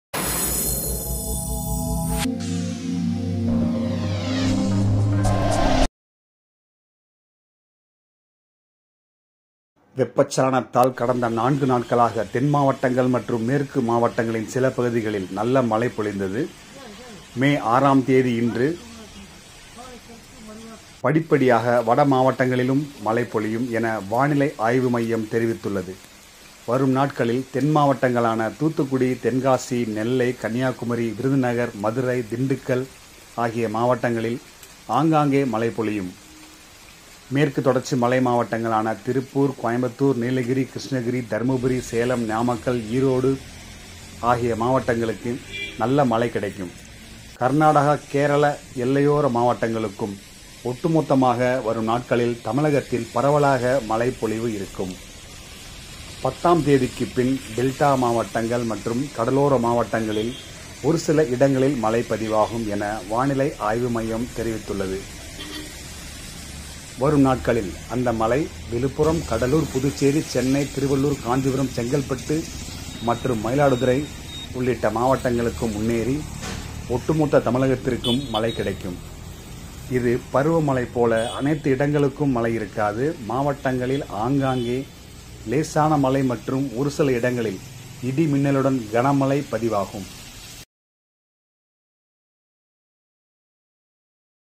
கோடை மழை|எங்கு எப்போது?live report
வெப்பச் சலனத்தால் தமிழகம் முழுவதும் பல பகுதிகளில் கோடை மழை பெய்து வருகிறது .குறிப்பாக தென் மாவட்டங்கள் ,மேற்கு மாவட்டங்கள் மற்றும் ஒருசிலர் வட மாவட்டங்களிலும் இந்த கோடை மழை வெப்பத்தை தணிக்கும்.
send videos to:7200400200